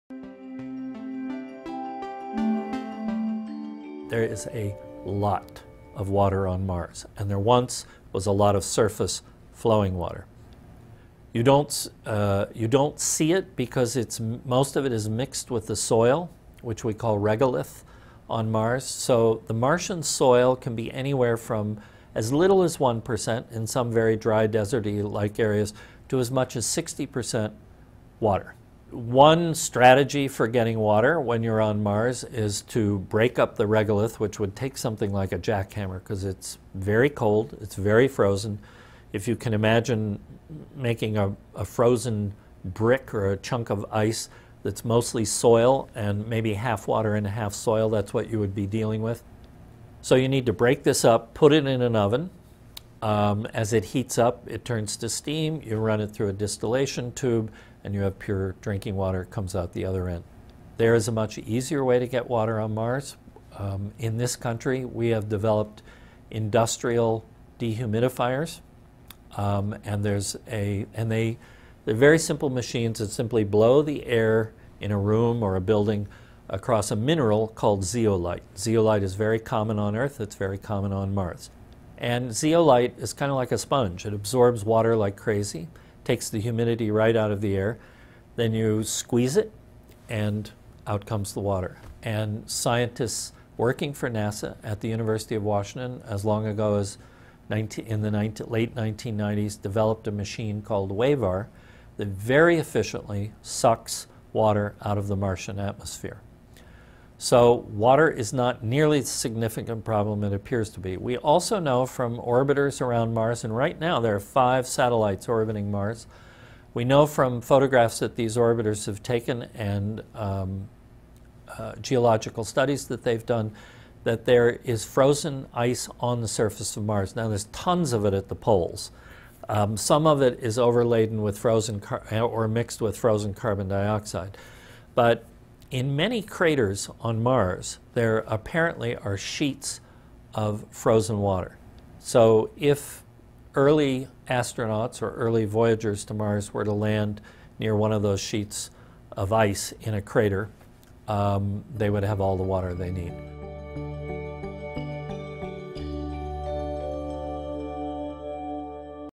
There's Plenty of Drinking Water on Mars | BEST OF 2015 | Big Think
There's Plenty of Drinking Water on Mars | BEST OF 2015
Stephen Petranek, author of How We'll Live on Mars, details several of the methods a future team of colonists could employ in order to amass a drinking water supply on Mars. There's plenty of water on the planet; the trick is extracting it from the soil and atmosphere. It's a relief that producing water won't be a major nuisance for the eventual Mars astronauts -- that whole "unlivable barren wasteland" is a whole other story.
STEPHEN PETRANEK:
Stephen Petranek’s career of over 40 years in the publishing world is marked by numerous prizes and awards for excellent writing on science, nature, technology, politics, economics and more. He has been editor-in-chief of The Miami Herald’s prestigious Sunday magazine, Tropic, as well covering a wide range of topics for Time Inc.’s Life magazine. His presentation, 10 Ways the World Could End, is one of the most original and most watched TED talks of all time. He is now the editor of Breakthrough Technology Alert, for which he finds the investment opportunities that create true value and move the human race forward. His new book is titled How We'll Live on Mars.
TRANSCRIPT:
Stephen Petranek: There is a lot of water on Mars and there once was a lot of surface, flowing water. You don’t see it because most of it is mixed with the soil, which we call regolith on Mars. So the Martian soil can be anywhere from as little as 1 percent in some very dry, deserty like areas to as much as 60 percent water. So one strategy for getting water when you’re on Mars is to break up the regolith, which would take something like a jackhammer because it’s very cold; it’s very frozen. If you can imagine making a frozen brick or a chunk of ice that’s mostly soil and maybe half water and half soil that’s what you would be dealing with. So you need to break this up, put it in an oven. As it heats up, it turns to steam. You run it through a distillation tube and you have pure drinking water that comes out the other end. There is a much easier way to get water on Mars. In this country, we have developed industrial dehumidifiers. And they’re very simple machines that simply blow the air in a room or a building across a mineral called zeolite. Zeolite is very common on Earth; it’s very common on Mars. And zeolite is kind of like a sponge. It absorbs water like crazy. Takes the humidity right out of the air. Then you squeeze it and out comes the water. And scientists working for NASA at the University of Washington as long ago as in the late 1990s developed a machine called WAVAR that very efficiently sucks water out of the Martian atmosphere. So water is not nearly as significant a problem than it appears to be. We also know from orbiters around Mars and right now there are five satellites orbiting Mars. We know from photographs that these orbiters have taken and geological studies that they’ve done that there is frozen ice on the surface of Mars. Now there’s tons of it at the poles. Some of it is overladen with frozen — or mixed with frozen carbon dioxide. But in many craters on Mars, there apparently are sheets of frozen water. So if early astronauts or early voyagers to Mars were to land near one of those sheets of ice in a crater they would have all the water they need.